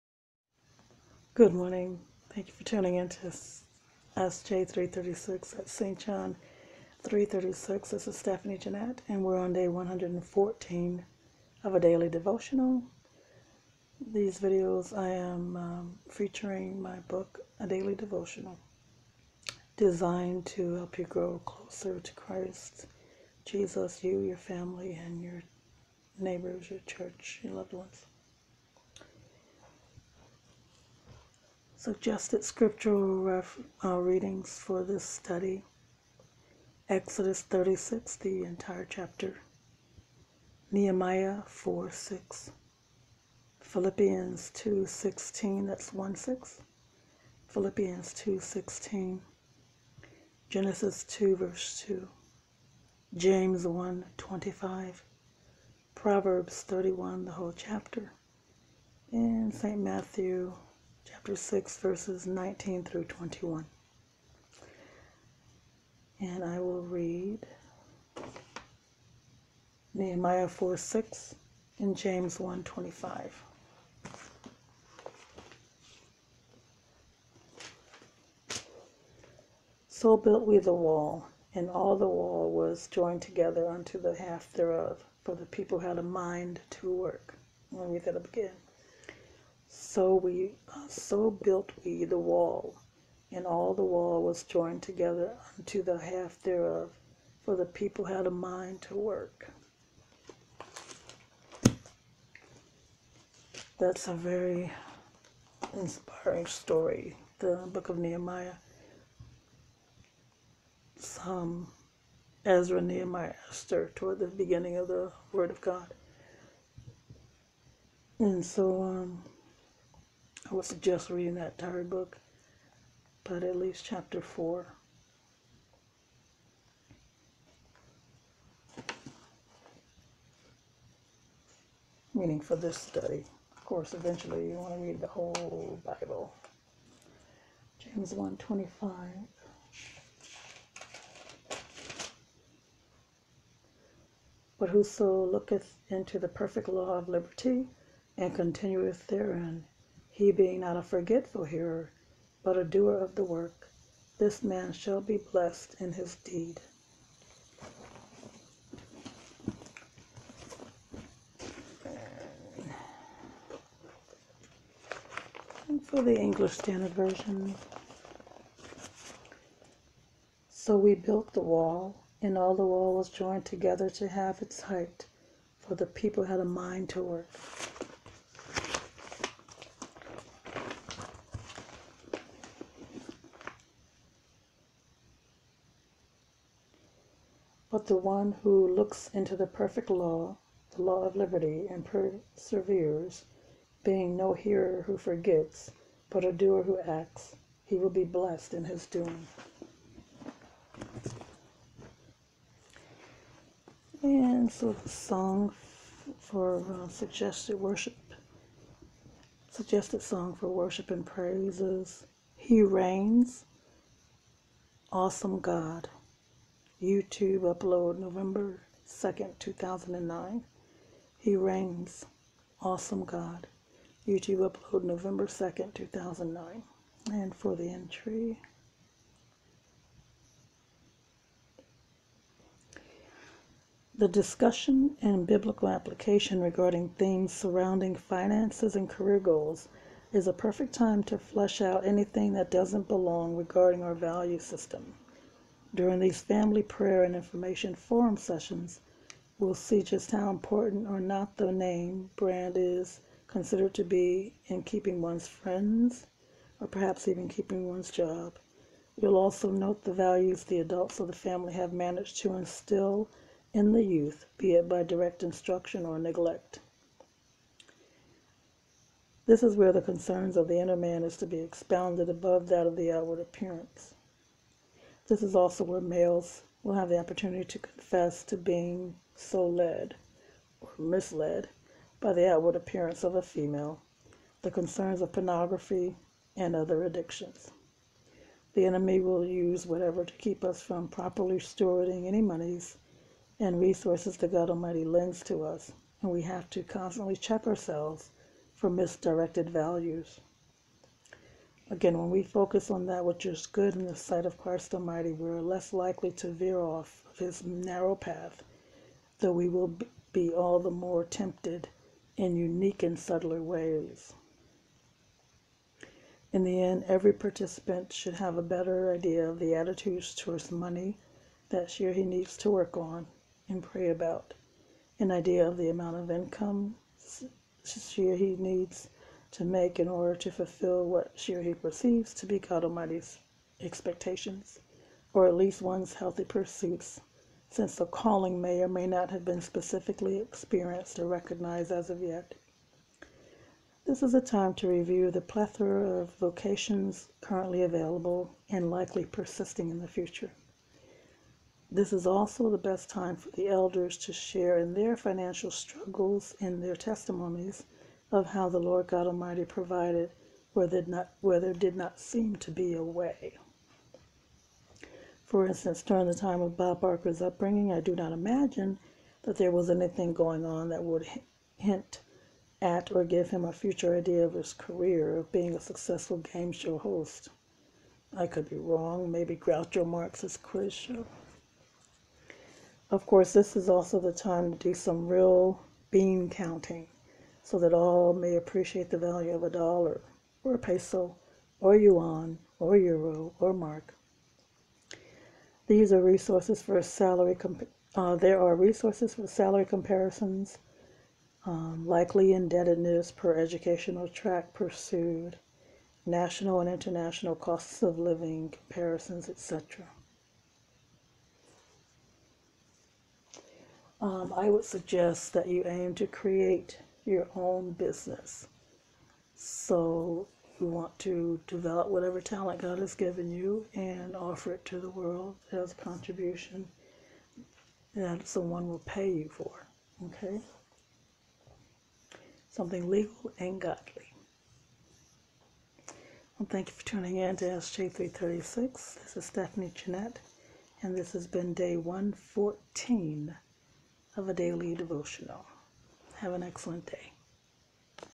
Sure & Secure. SJ336 A.D.D.: A Daily Devotional, Day 114
We can plan for the future.  We're to still keep ChristJESUS Foremost and Center in our lives.  And if we are doing so, then our plans for the future are centered around Him and His Truth, as well.

Folk who don't want you to plan for yourself (and with your family, also  in ChristJESUS) are folk who want to have access to your time, talent, and/or money without boundaries.  These are folk who donot have your best interests in mind.

Sure,  GOD  Almighty  may flip your plans seemingly out of the blue but He will honor your diligence and holiness.  Don't be surprised if 20 years down the road He brings the use of those skills even more relevant to your daily life and/or Calling.  The good thing about it all is that because you have accepted ChristJESUS as your LORD and SAVIOUR, your life and future are secure.  Don't waste your life.  Don't waste your anointing.  Don't waste time being afraid to answer His Call.

It is important to value GOD Almighty's Will fruition on earth.  It is also important to experience as many vocational options as possible, receiving exposure to some aspect of them from a very young age.  Yes, some of us know 'right off the bat,' so to speak, from toddler age what we want to do.  That's awesome.  Even at that age, be sure to start doing things related to securing that aim.  Don't leave it as some nebulous, foggy reach but rein it on in.  Time flies.